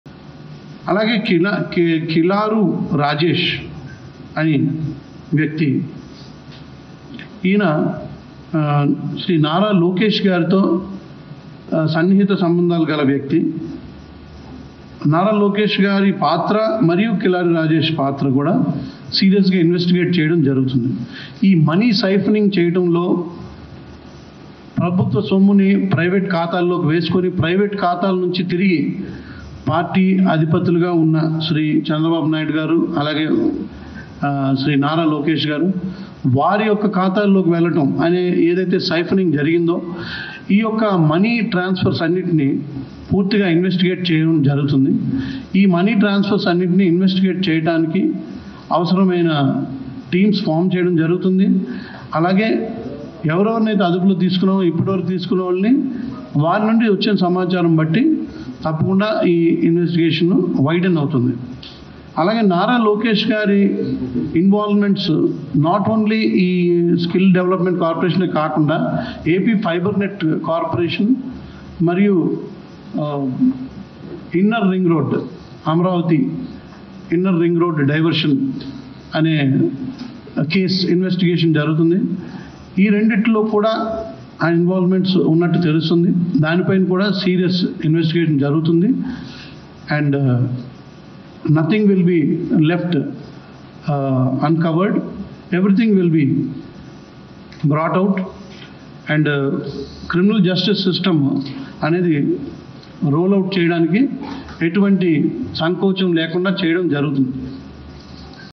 మా నెక్స్ట్ టార్గెట్ లోకేష్| CID DG Sanjay Sensational Comments on Lokesh| Chandrababu | Jaitra News
@jaitranews is an official Youtube Channel which will constantly keep you updated with the latest breaking news, trending topics, entertainment videos, viral  videos,